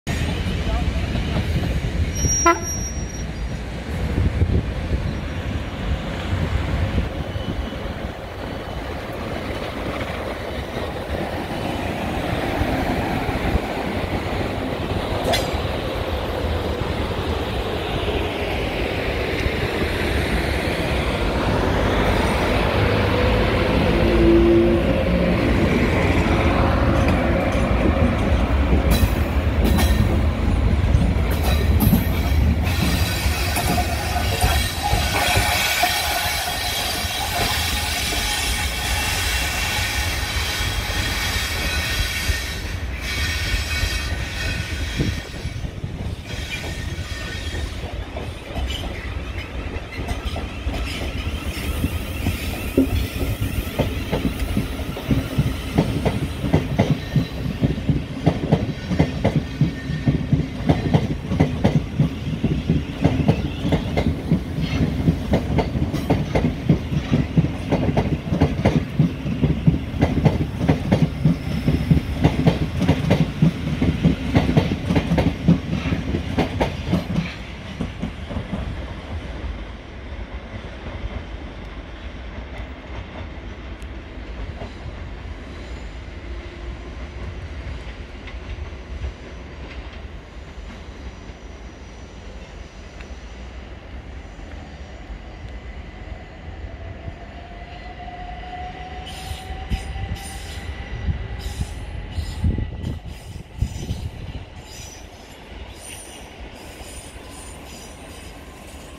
73 212/73 213 departing Clapham Junction Platform 3 with 0Y69 to Tonbridge with 69001
73 212/73 213 departing Clapham Junction Platform 3 with train 0Y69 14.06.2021 to Tonbridge with 69001. I had just got out of the 69 to get another connection. 73's depart Clapham at 1938 Platform 3 on electric power with 69001 hauled dead to Tonbridge from Eastleigh. My film.